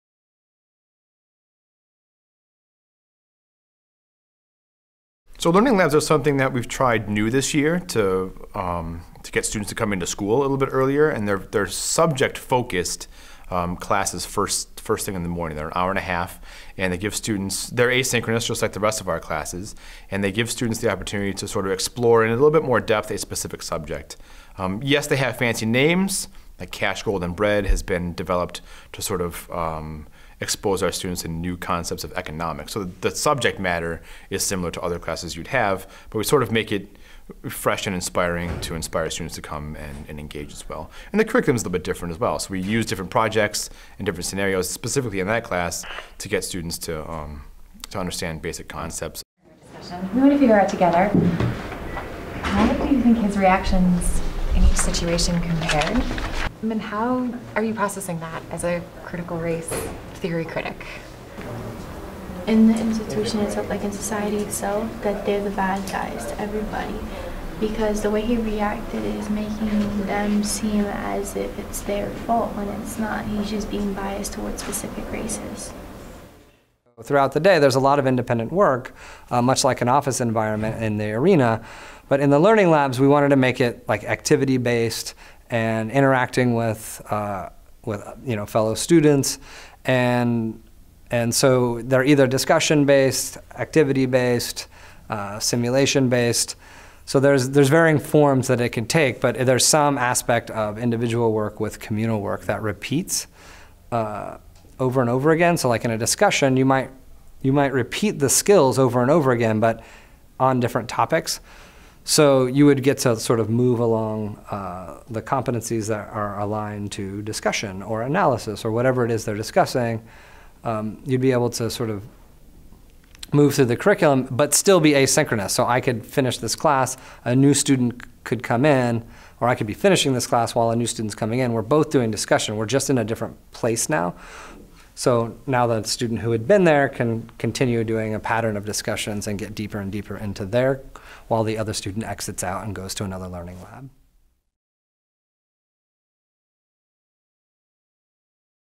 Learning Labs at Bronx Arena
Educators at Bronx Arena discuss subject-specific activity-based classes called learning labs. Classroom footage is also featured.